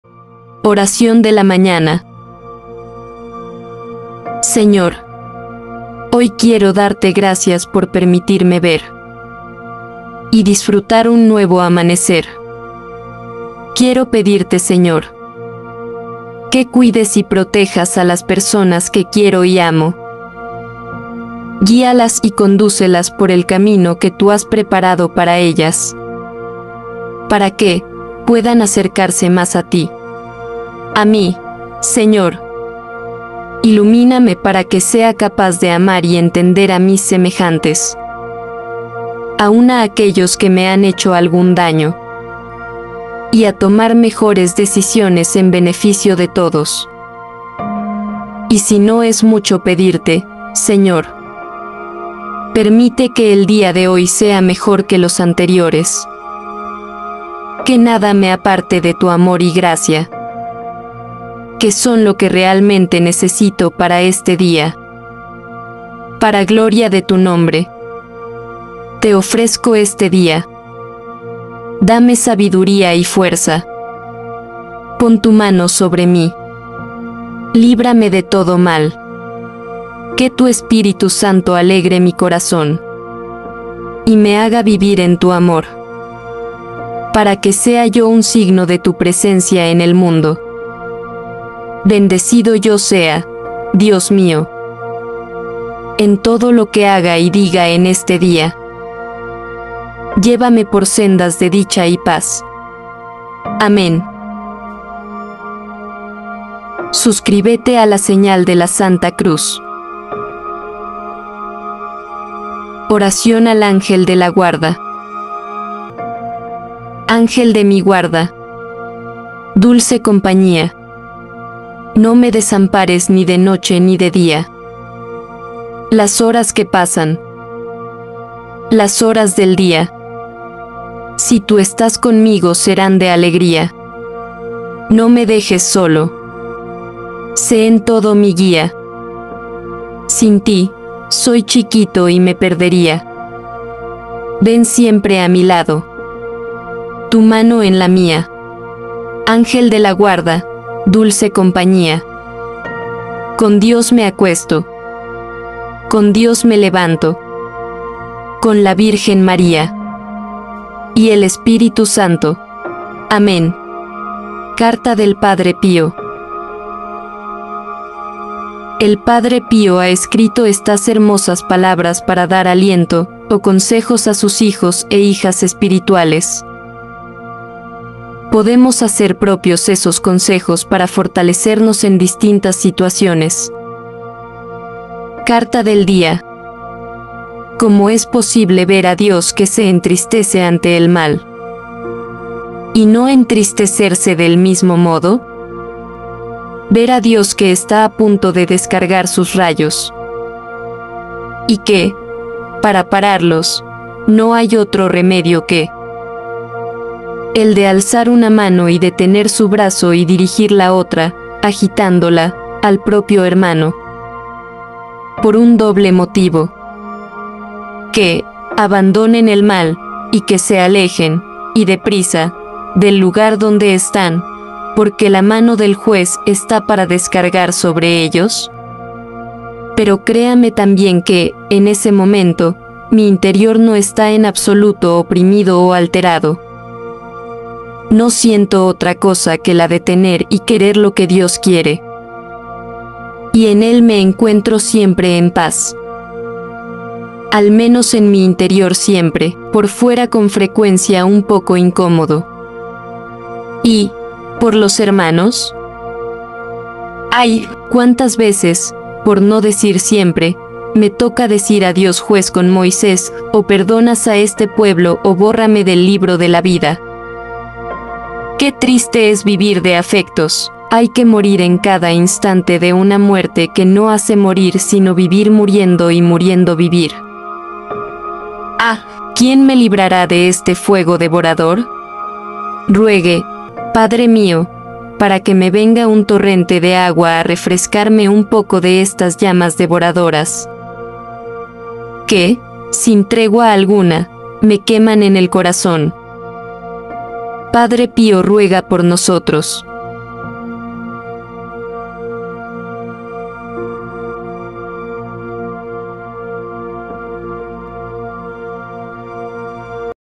Padre Pío: Carta de Hoy 21 de Noviembre 💌 y Oración de la Mañana 🙏
Carta del Padre Pío y Oración de la Mañana 🙏